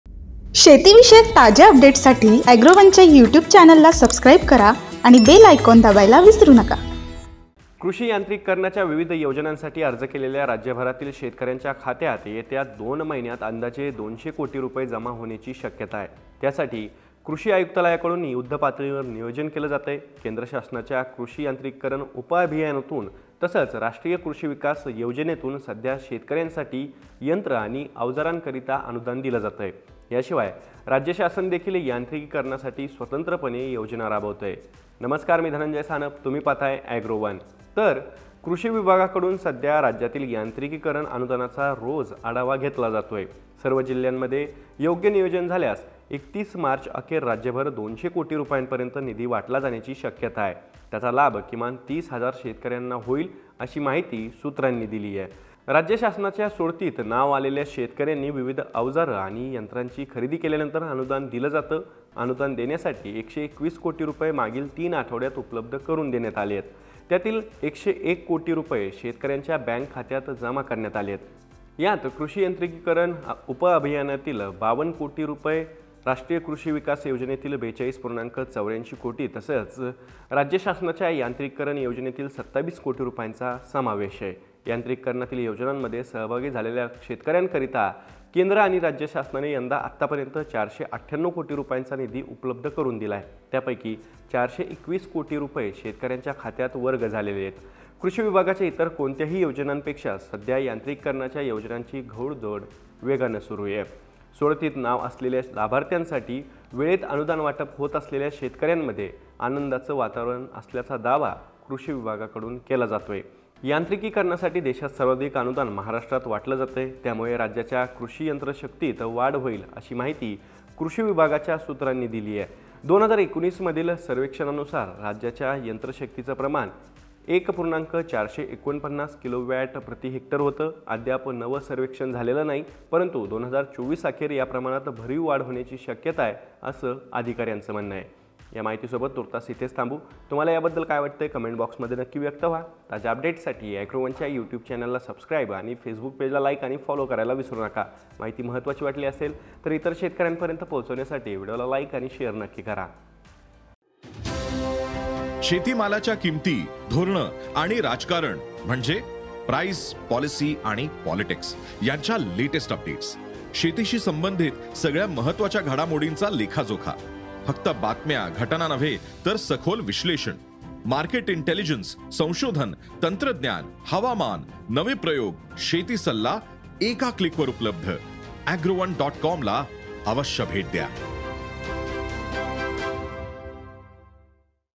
Agricultural Mechanization Subsidy : २०० कोटी रुपये जमा होण्याची शक्यता | ॲग्रोवन
यांत्रिकीकरणातील योजनांमध्ये सहभागी झालेल्या शेतकऱ्यांकरीता केंद्र व राज्य शासनाने यंदा आतापर्यंत ४९८ कोटी रुपयांचा निधी उपलब्ध करून दिला आहे. त्यापैकी ४२१ कोटी रुपये शेतकऱ्यांच्या खात्यात वर्ग झालेले आहेत.

The central and state governments have provided funds of Rs 498 crores for the farmers who have participated in the mechanization schemes so far this year. Out of which Rs 421 crores have been deposited in farmers' accounts.